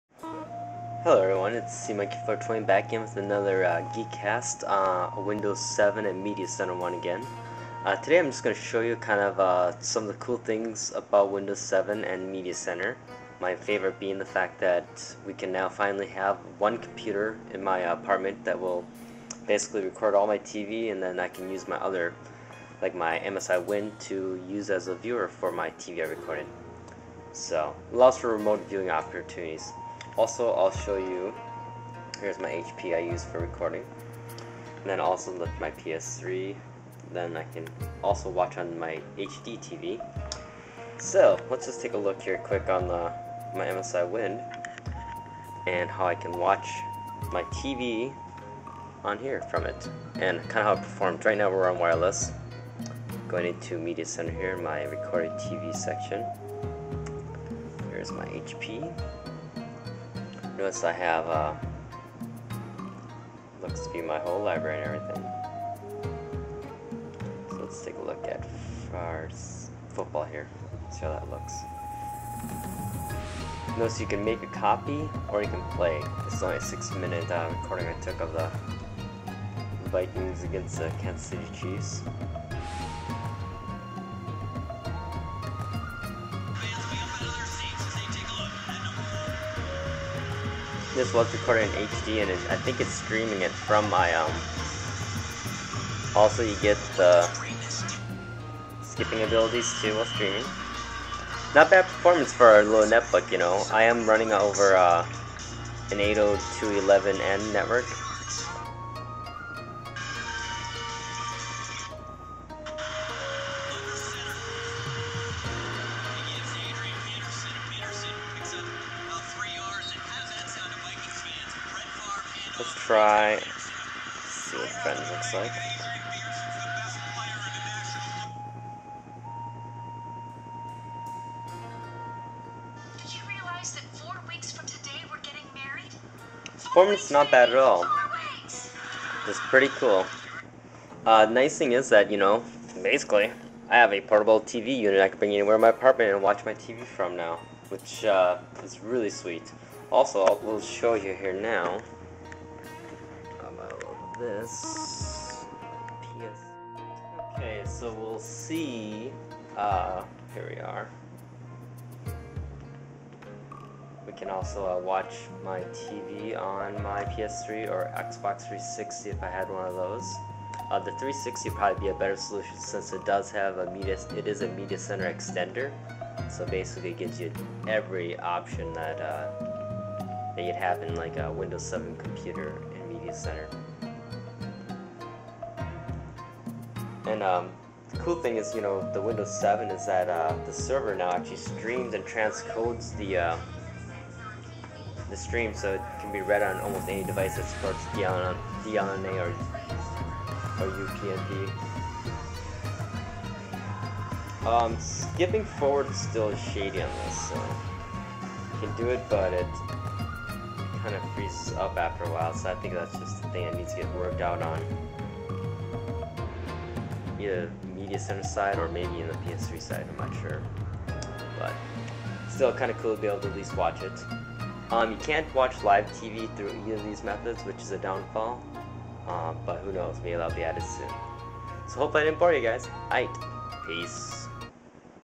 Win7 and Sharing TV
just a quickie demo of windows7 and viewing recorded tv on different win7 laptops (msi wind in video) and on the ps3.